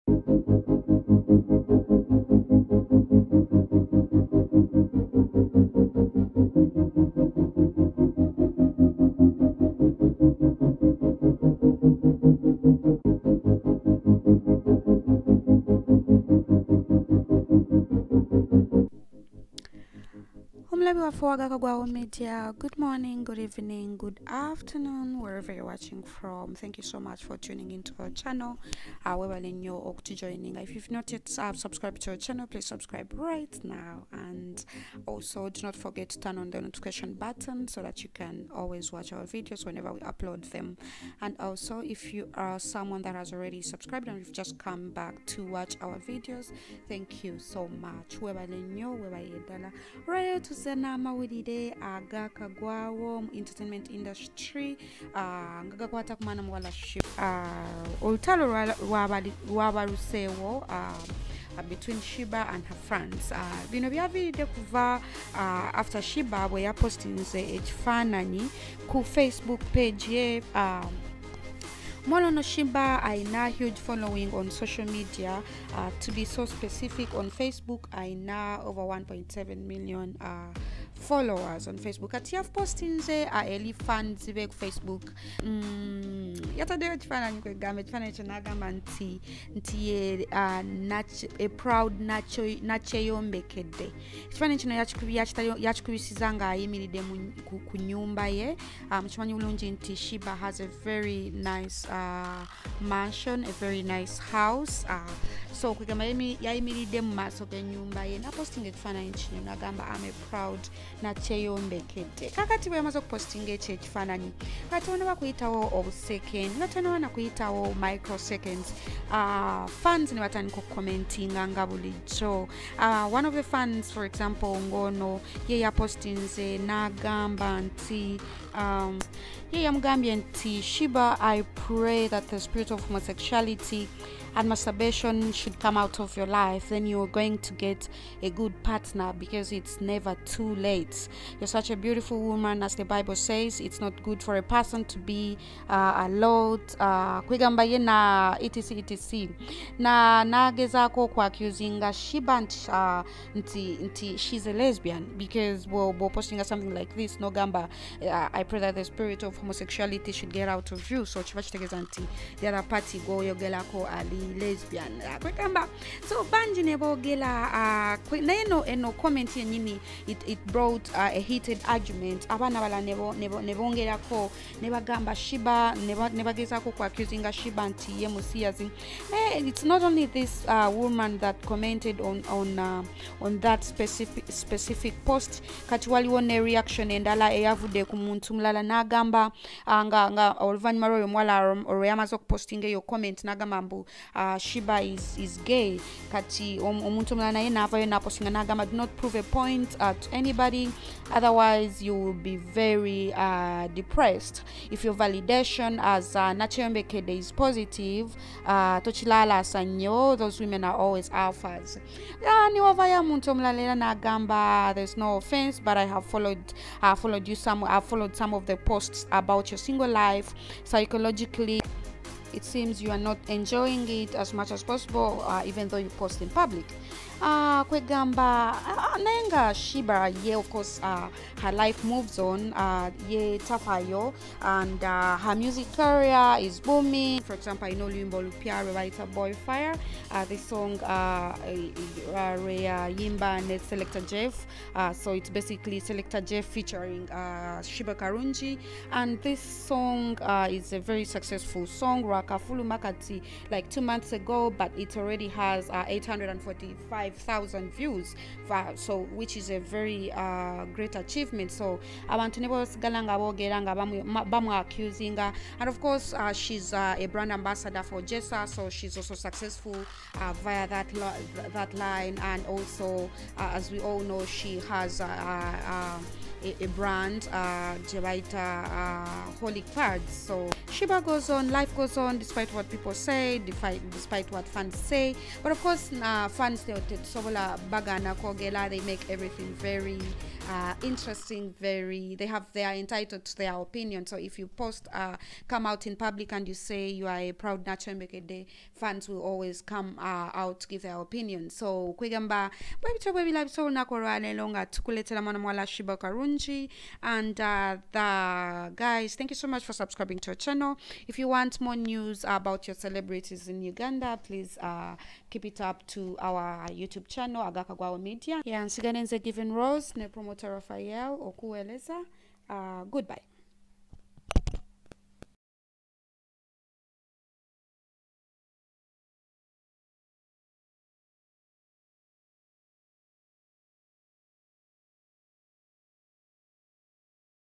Sheebah Karungi Ba Fan Bamulangidde Obusiyazi
This Video is  a review  about Sheeba Karungi as she falls out with fans who accused her of being Lesbian.
What till the end to see what transpired.

Thanks For Your Support 🙏🙏 Your One Sub Means A lot To Us.